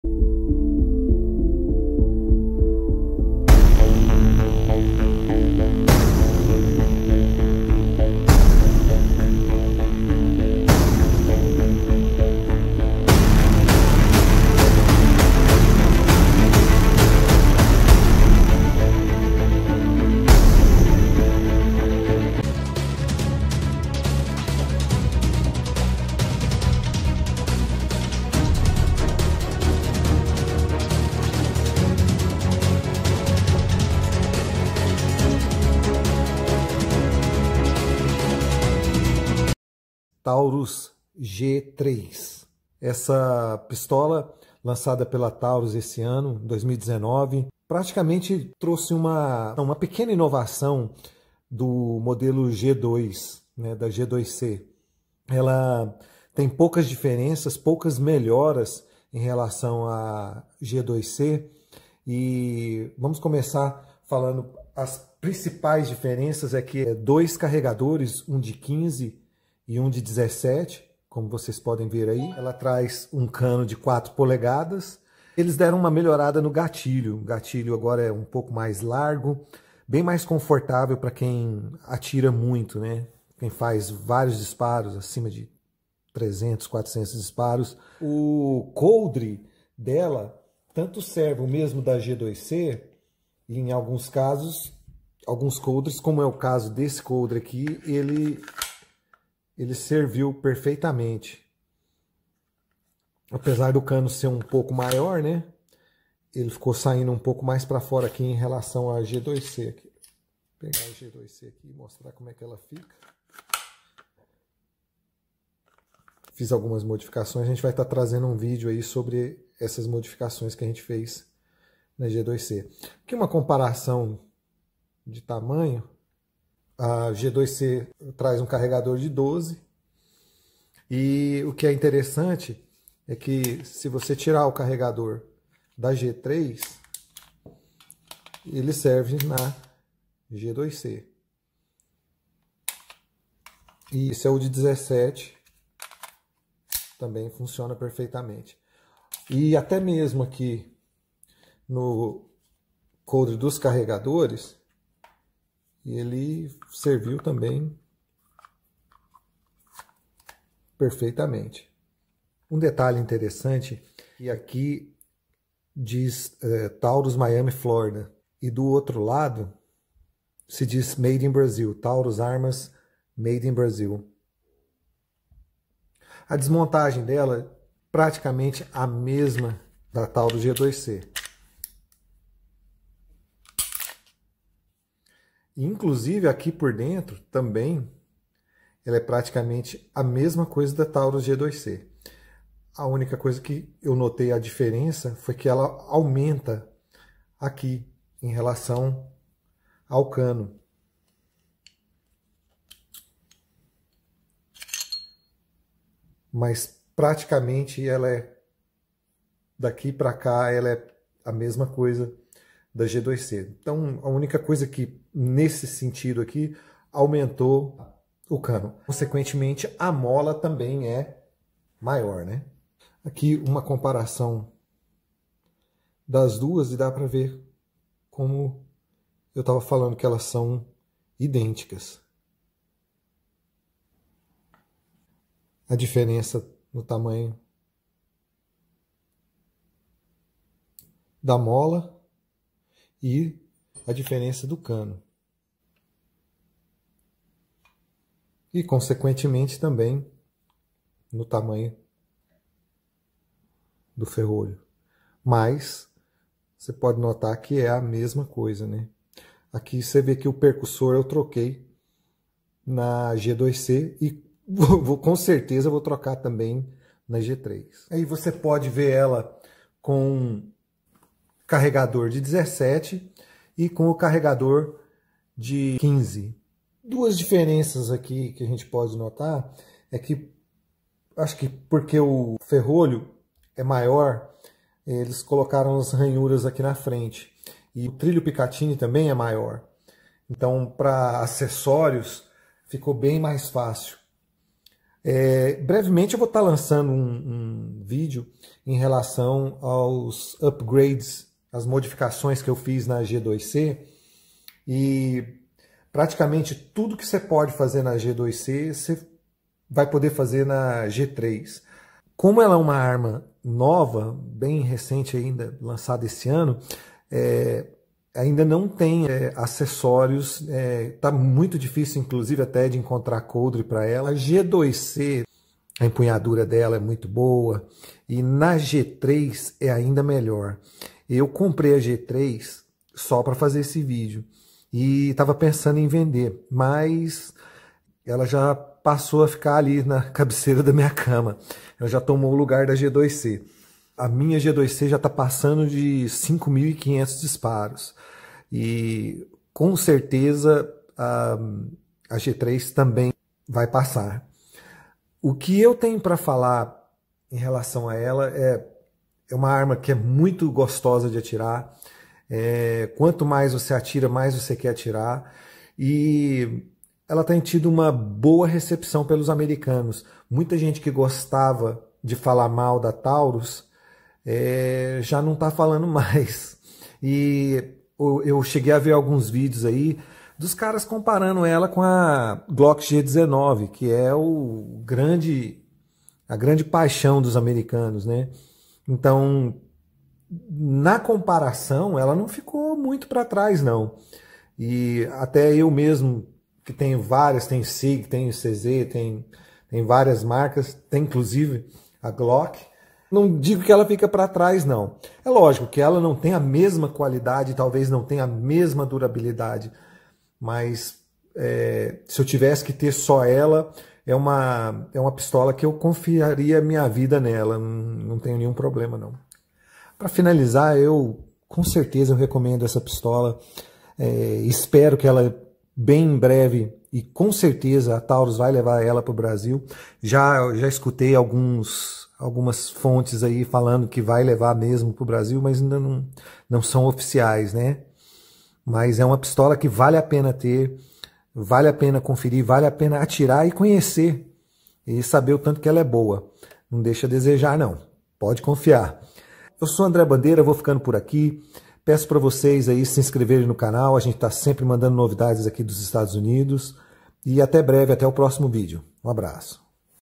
LANÇAMENTO NOVA Pistola Taurus G3 vs G2C CALIBRE 9MM 🇧🇷🇺🇸
Review da pistola Taurus G3 em português

André Bandera é instrutor de tiro nos Estados Unidos
Em breve estará trazendo cursos da SWAT para o Brasil